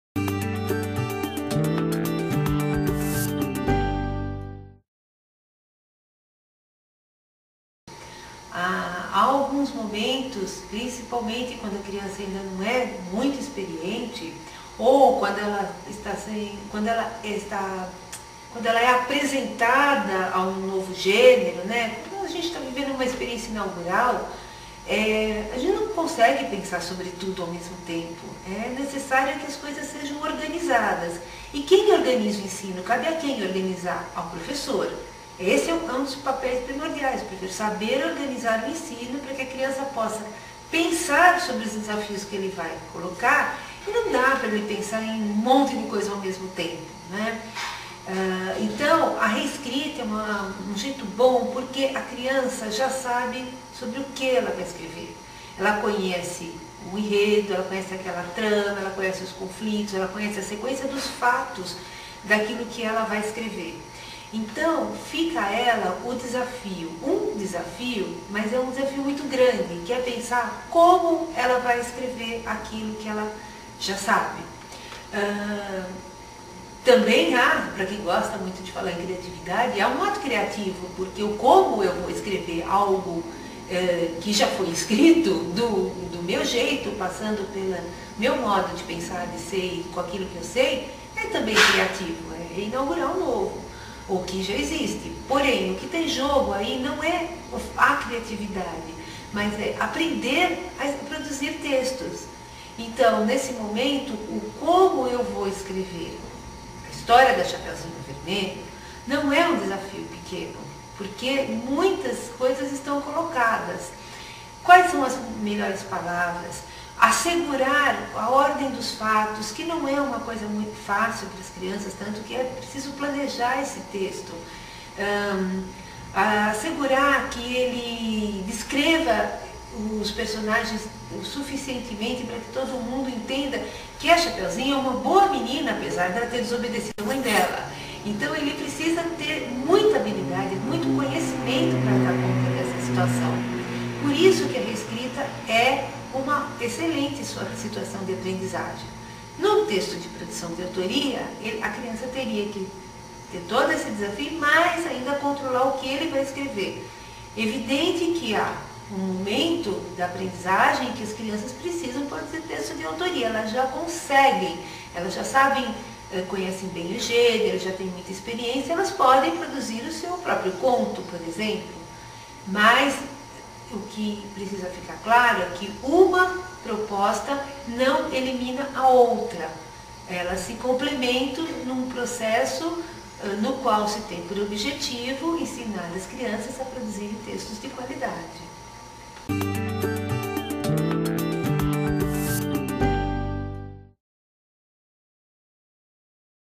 Reescrita no percurso de autoria
Débora Rana, professora e formadora do Instituto Avisa Lá, explica como um percurso de autoria pode ser construído a partir da reescrita textos conhecidos pelos alunos.